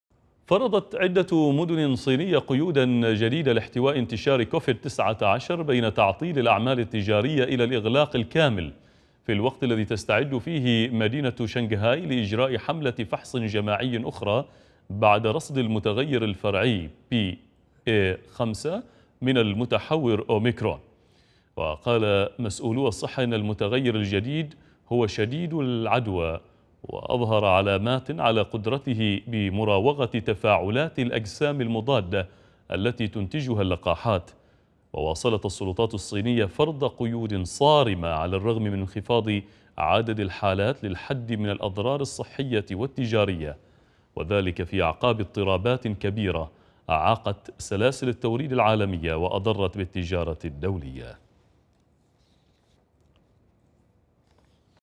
الصين تفرض قيودا جديدة في مدنٍ عدة لاحتواء انتشار كوفيد-19 بعد رصد متحور فرعي جديد من أوميكرون
السلطات الصينية تفرض قيودا جديدة في مدنٍ عدة لاحتواء انتشار كوفيد-19 بعد رصد المتحور الفرعي أوميكرون بي إيه فايف